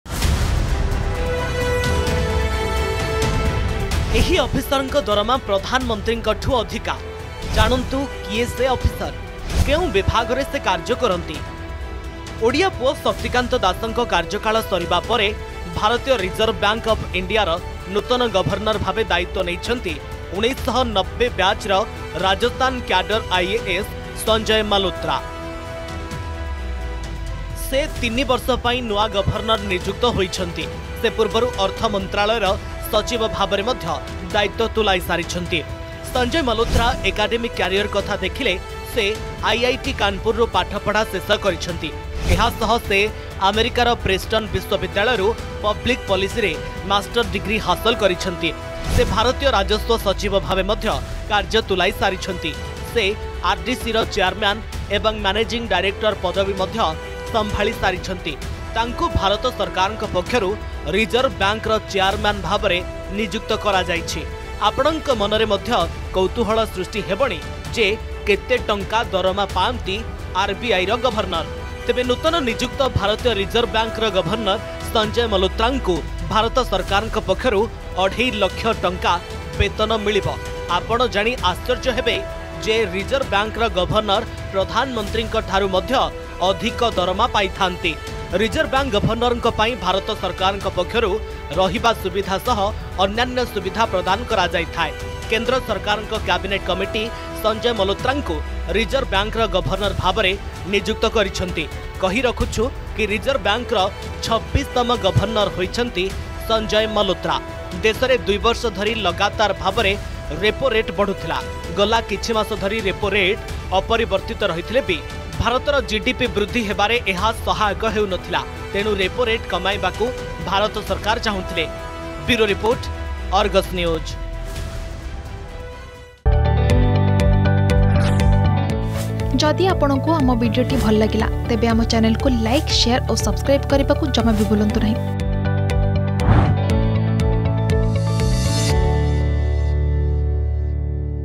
ଏହି ଅଫିସରଙ୍କ ଦରମା ପ୍ରଧାନମନ୍ତ୍ରୀଙ୍କଠୁ ଅଧିକା ! Salary Of RBI Governor
ଏହି ଅଫିସରଙ୍କ ଦରମା ପ୍ରଧାନମନ୍ତ୍ରୀଙ୍କଠୁ ଅଧିକା । ଜାଣନ୍ତୁ କିଏ ସେ ଅଫିସର । କେଉଁ ବିଭାଗରେ ସେ କାର୍ଯ୍ୟ କରନ୍ତି  ।